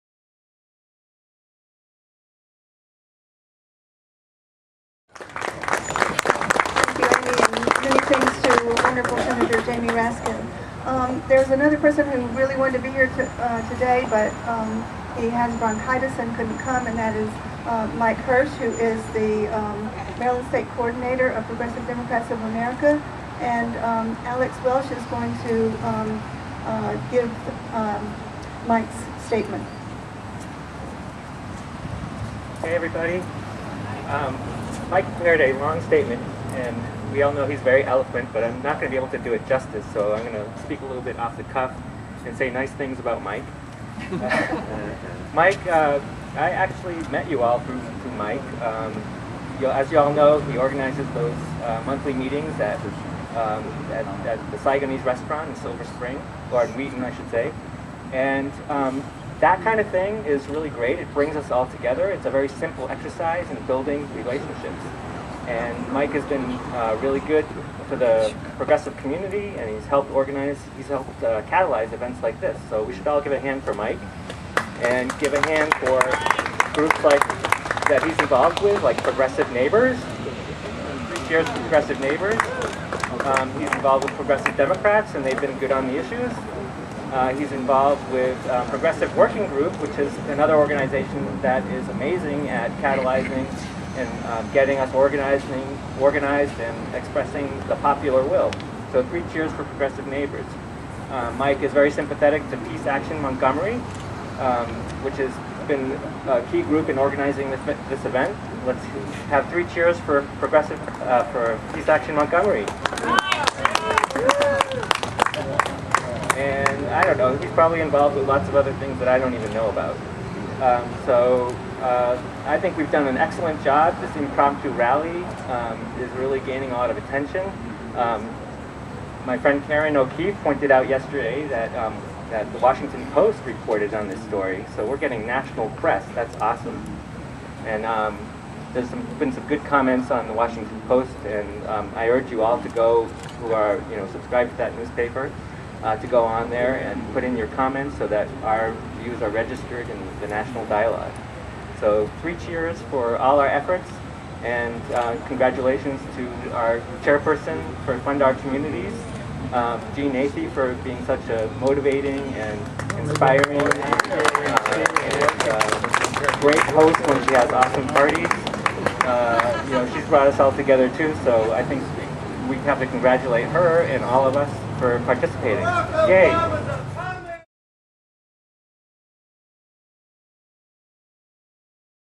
Fund Our Community's Alex Welsch speaks at 10/11/11 protest press conference
On Tuesday, October 11, 2011, "Fund Our Communities" coalition members held a rally and press conference on the stairs of the Montgomery County Council building.   The event was held to protest Council's disappointing decision to yield to Lockheed Martin lobbying and shelve consideration of the "Resolution Supporting Federal Budget Reprioritization" advocated by FOC.  Links: